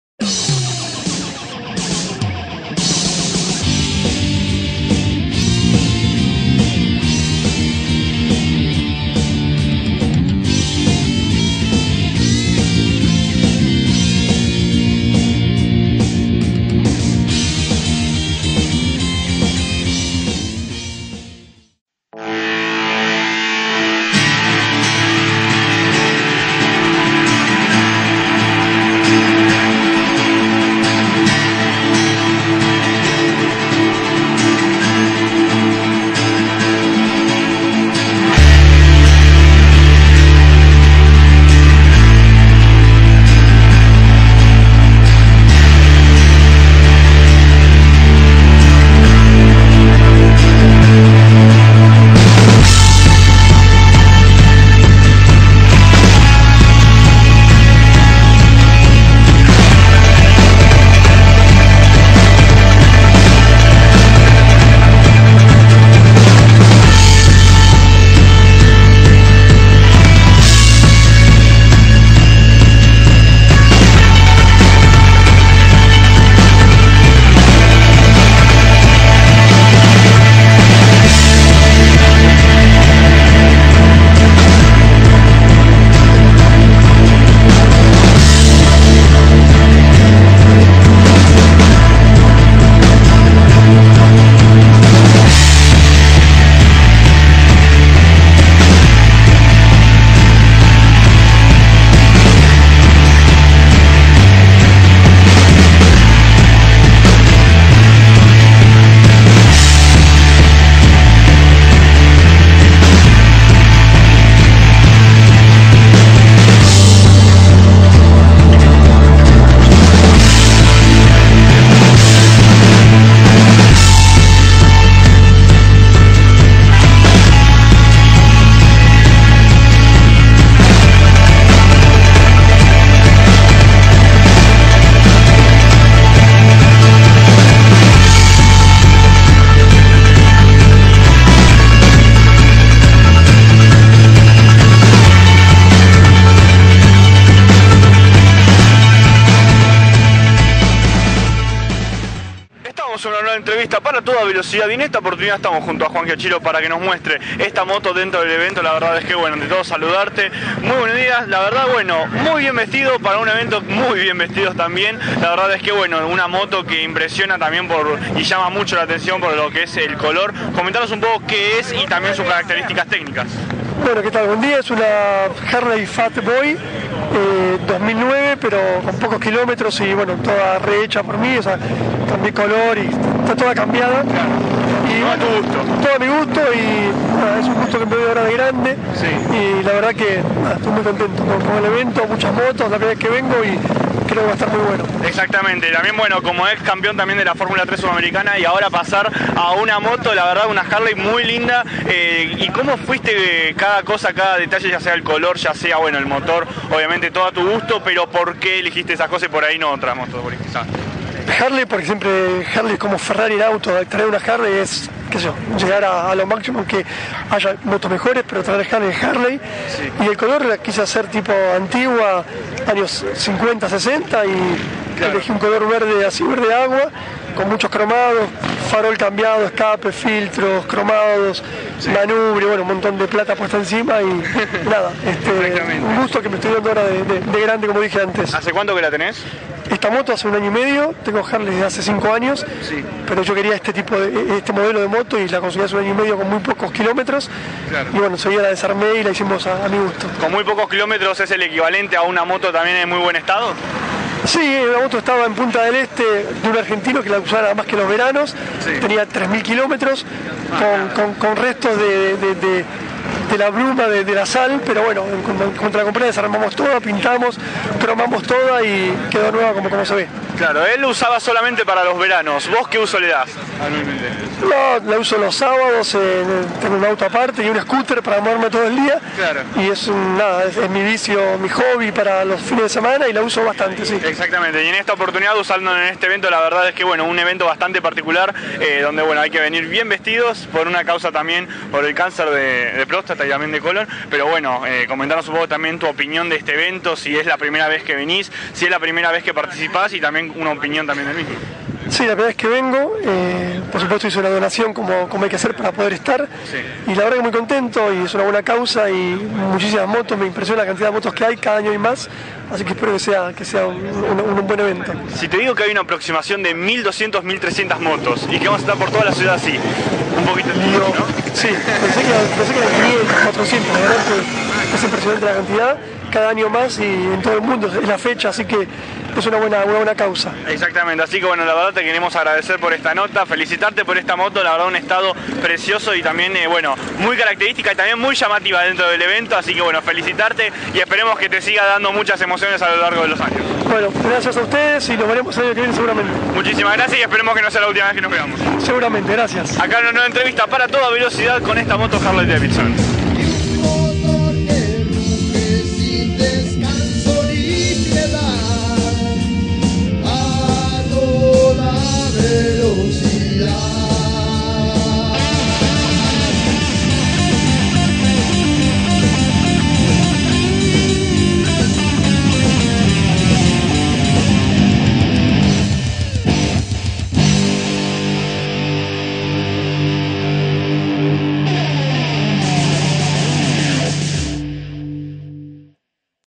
The Distinguished Gentleman's Ride - Harley Davidson de Juan Giacchino - Parte 2 - 25/09/2016.
En esta segunda parte te dejamos imágenes del evento
con la participación de mas de 1000 motos y la entrevista
al campeón de la F3 Sudamericana junto a su Harley.